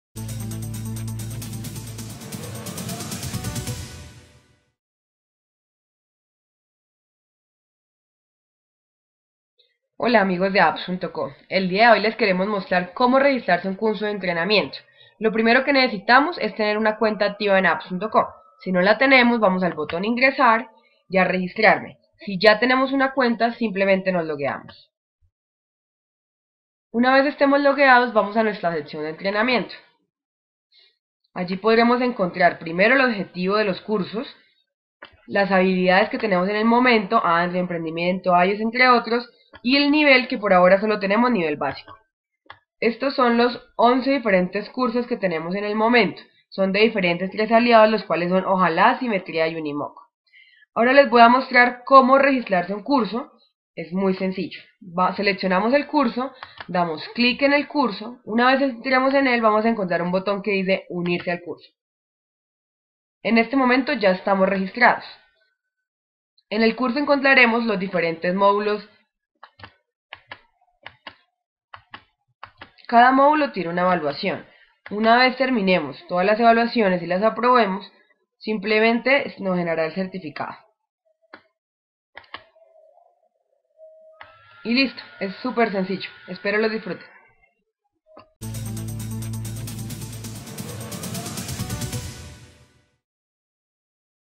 Inscripción a cursos de entrenamiento de Apps.co
El paso a paso del proceso de inscripción a la oferta de cursos de entrenamiento de apps.co.